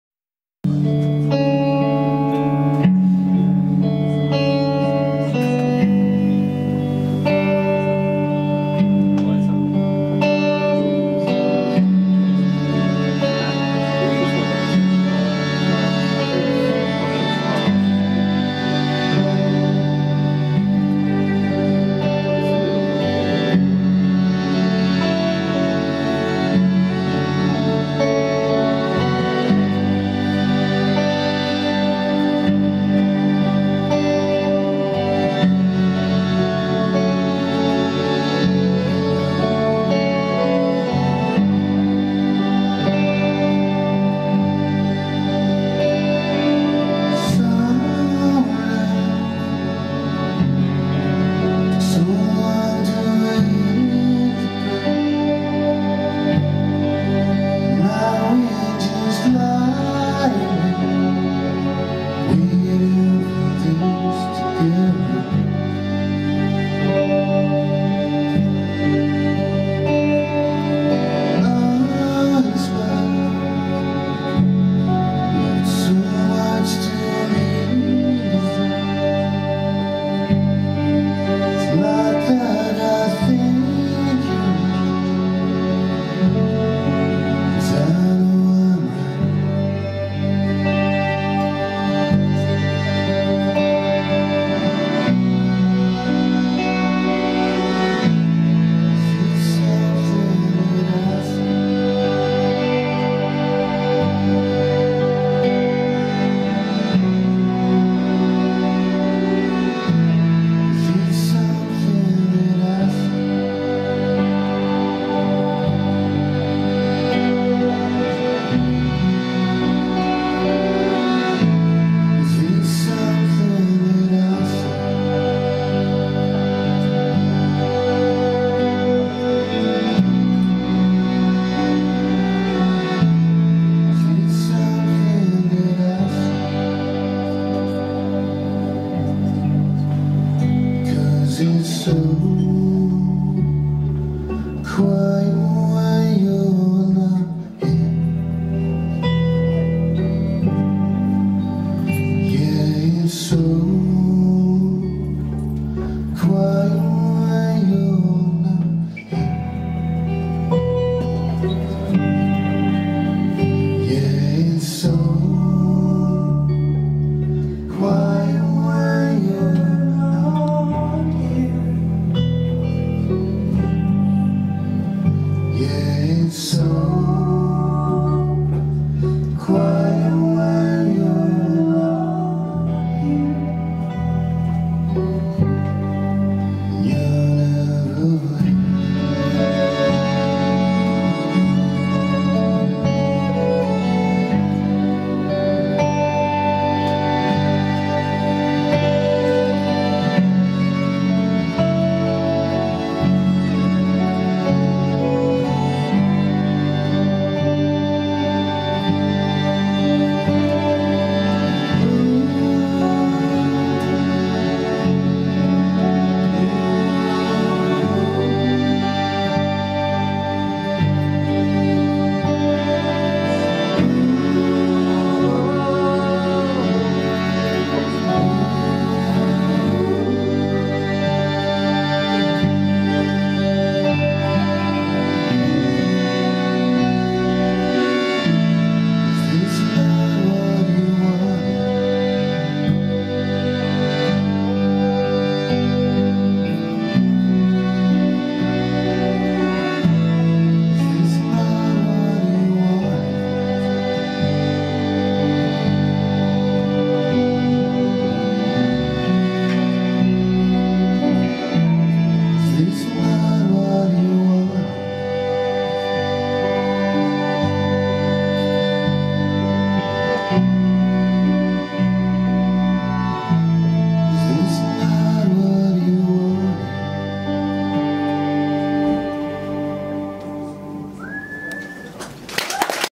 eagleowl vs woodpigeon
Record Store Day, Saturday 20th April 2013

The Last Word Saloon, in association with VoxBox Records

Eagleowl and Kid Canaveral